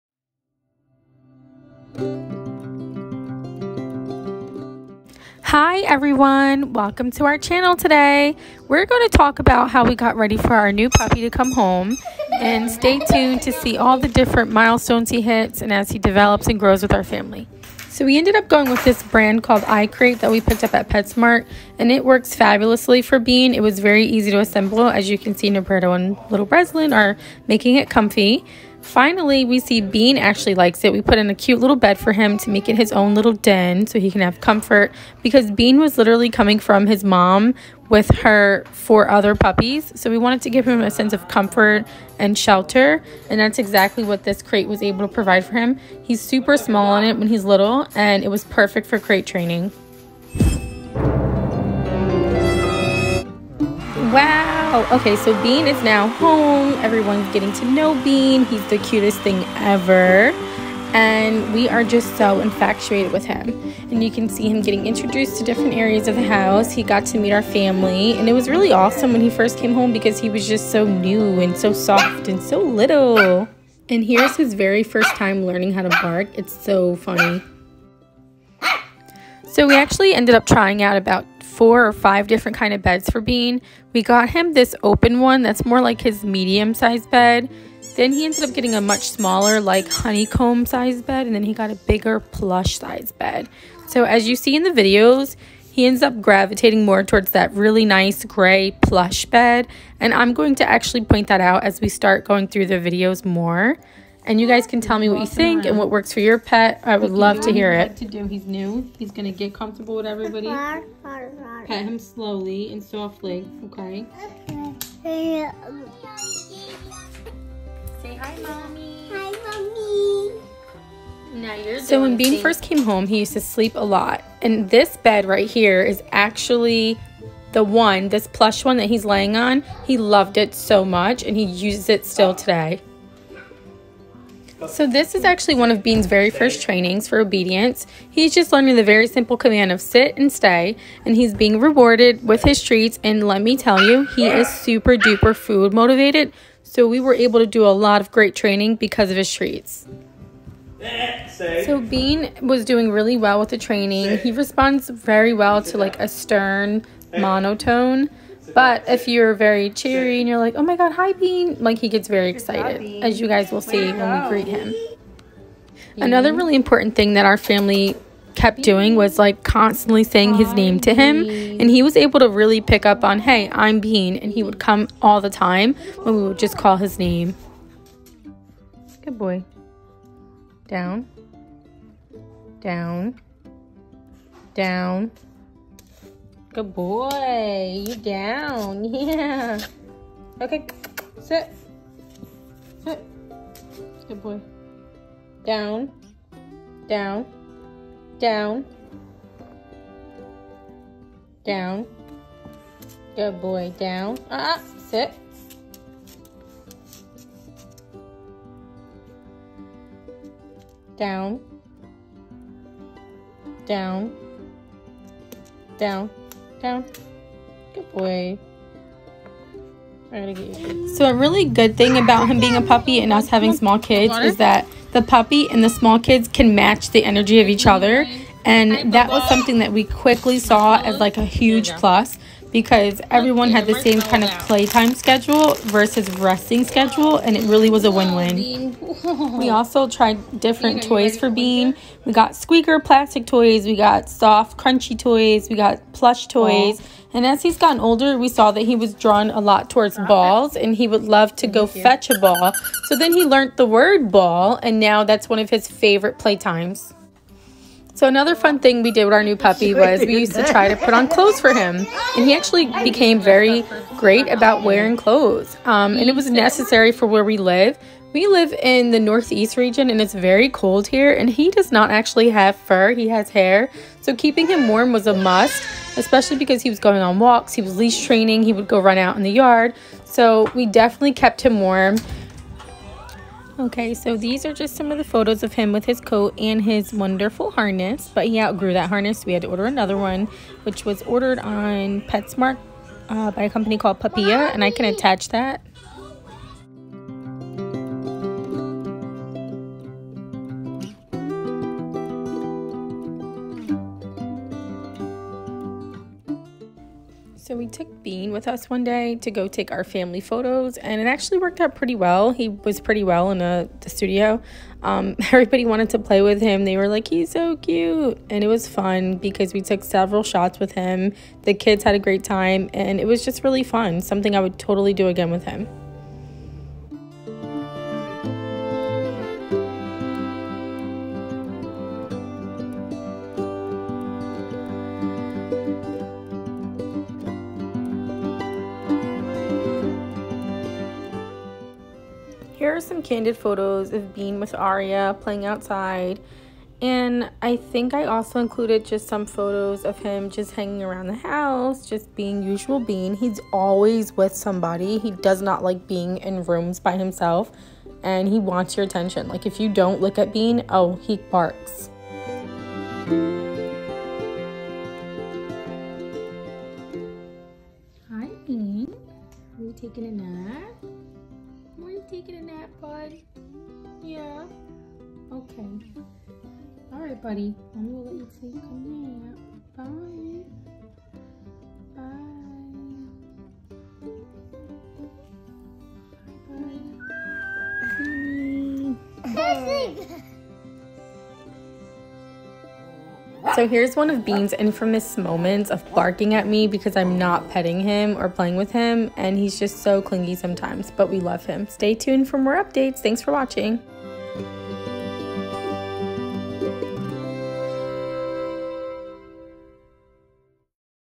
Meet our Furbaby Bean! He's a Shorkie Mix
Say Hello to Bean! He's our new fur baby and we are sharing how we get ready to bring him home. We will show how he has grown from an 8 week old puppy to his present age at 10 months.